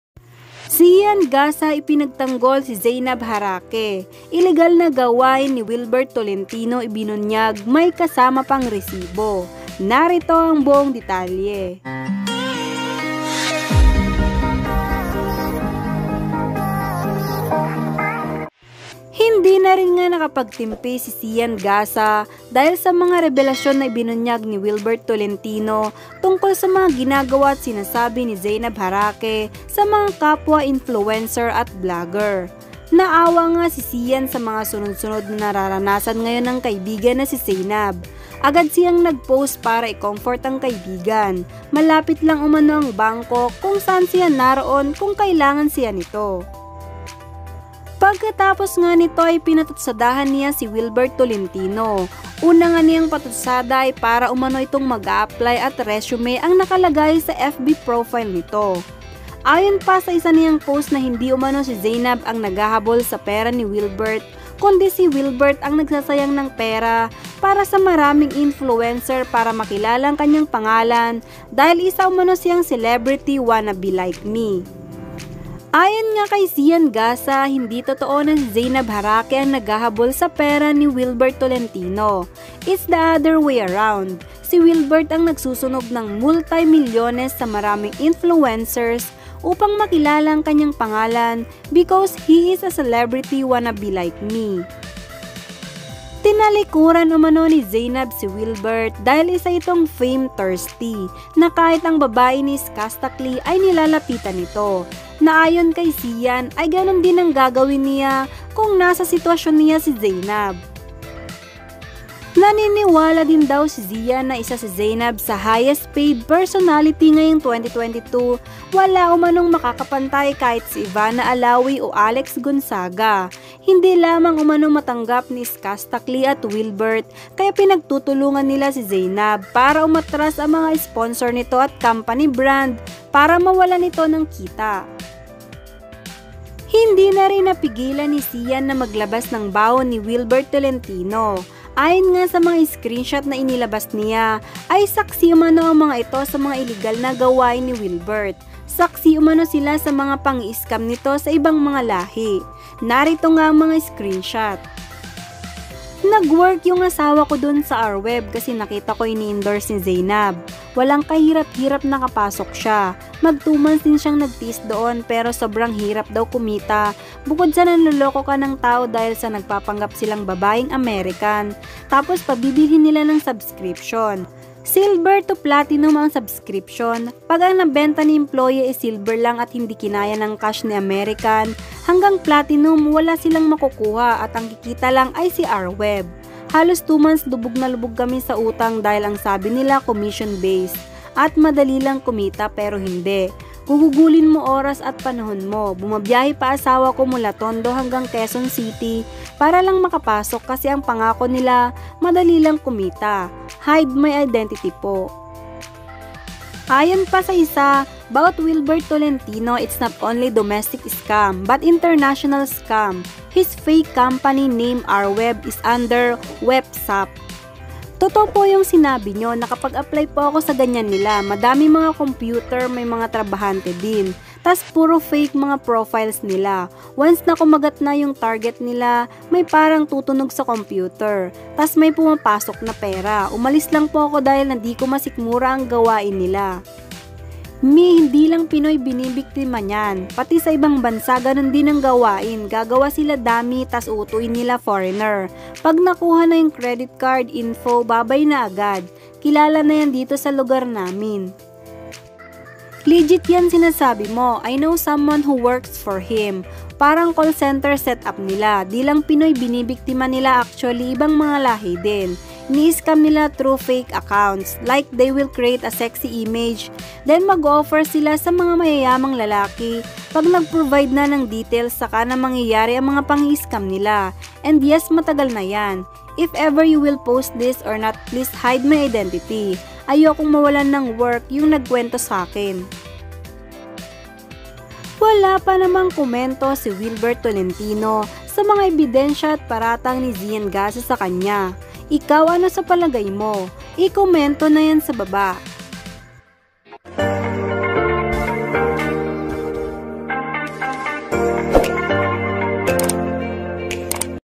Xian Gaza naglabas ng ebidensya at Screenshot sa ILL*GAL at SC*AM NA COMPANY ni Wilbert Tolentino
Xian Gaza naglabas ng Screenshot na ebidensya sa ILL*GAL at SC*AM NA COMPANY ni Wilbert Tolentino